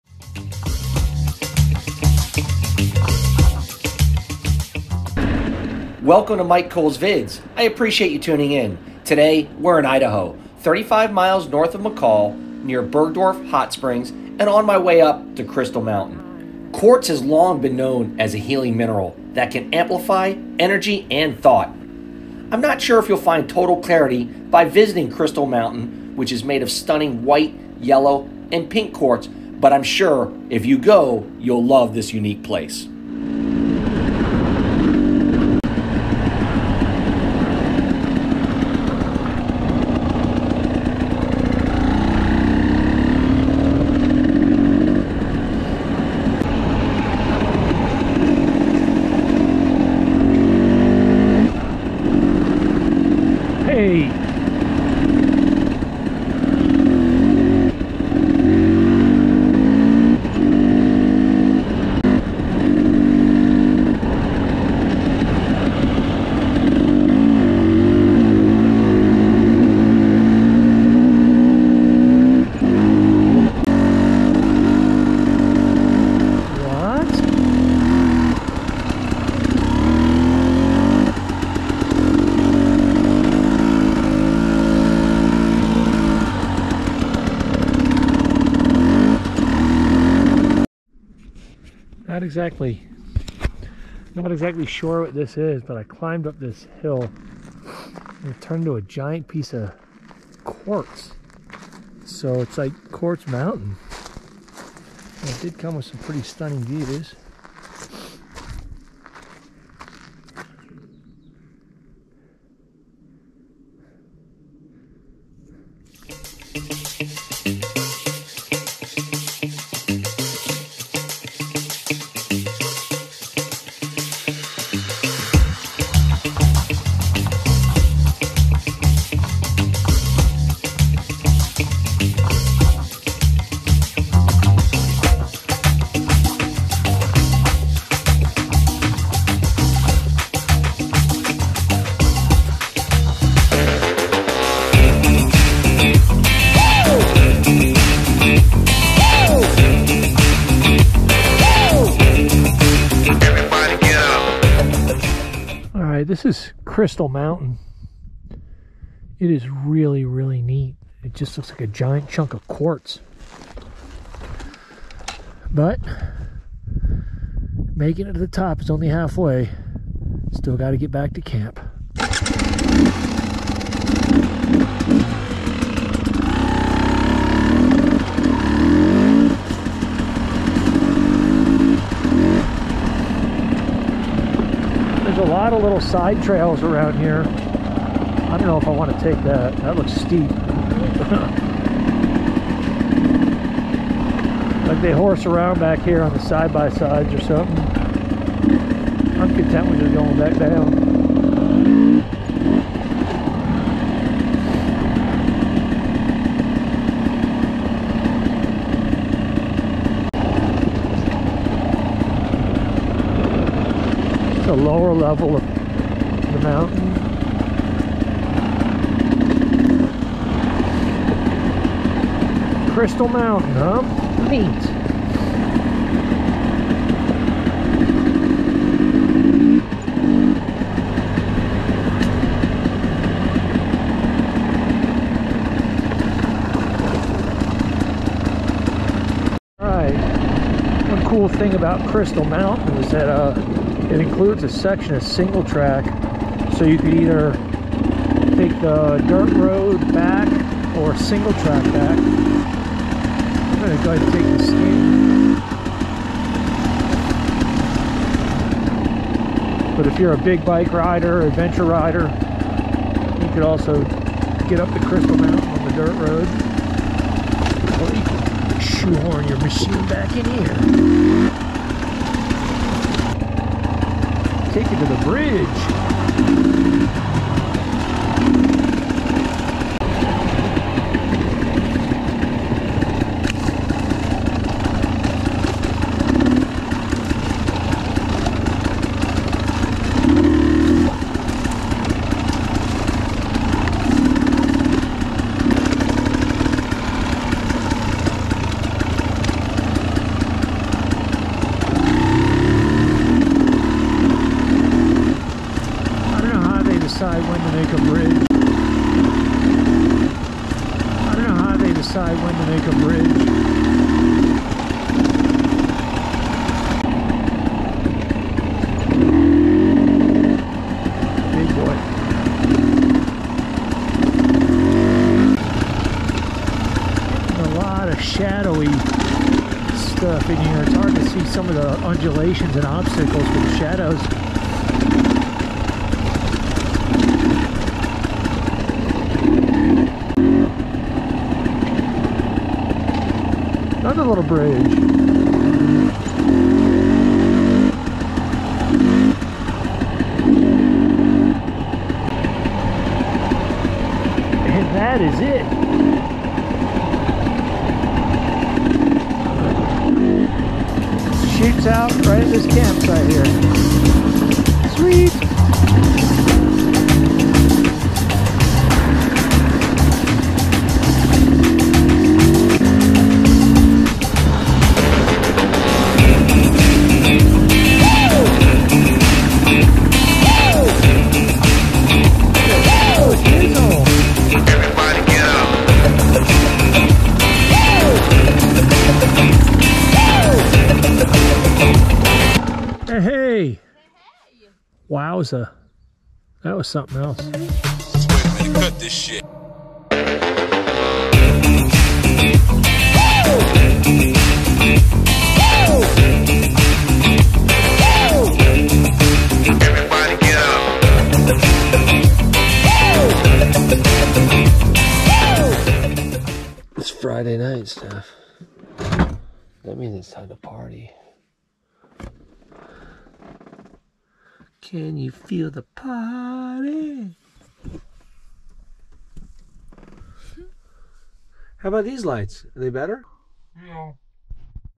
Crystal Mountain, dual sport Idaho!
A mid July ride to Crystal Mountain 35 miles north of McCall Idaho!